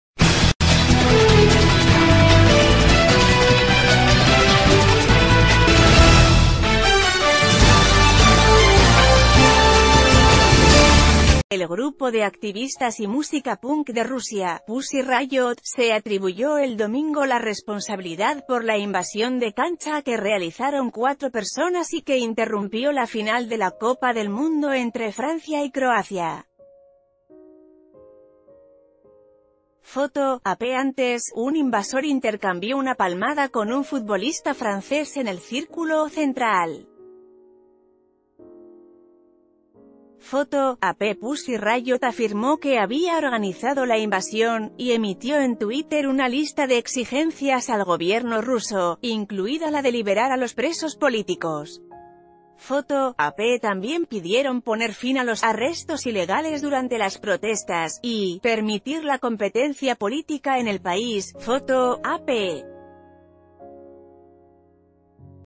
¿Quiénes son los intrusos que ingresaron al campo durante la final?
El grupo de activistas y música punk de Rusia, Pussy Riot, se atribuyó el domingo la responsabilidad por la invasión de cancha que realizaron cuatro personas y que interrumpió la final de la Copa del Mundo entre Francia y Croacia. Foto: AP Antes, un invasor intercambió una palmada con un futbolista ...